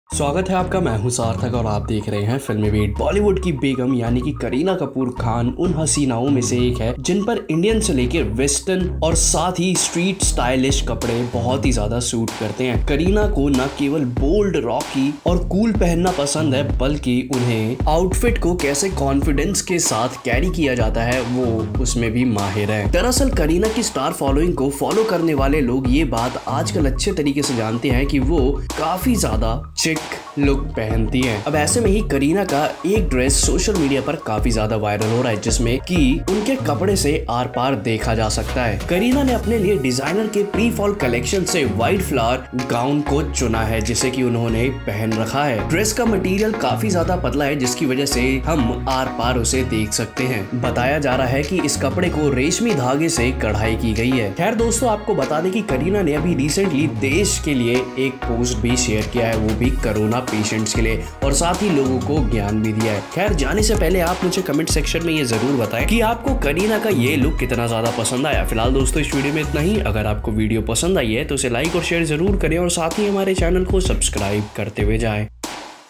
Kareena Kapoor Khan का आरपार दिखने वाली Wedding Dress Social Media पर हुई Viral! | FilmiBeat
Kareena Kapoor Khan's Wedding Dress Went Viral On Social Media, Check Out Photos? Know more about that in this video.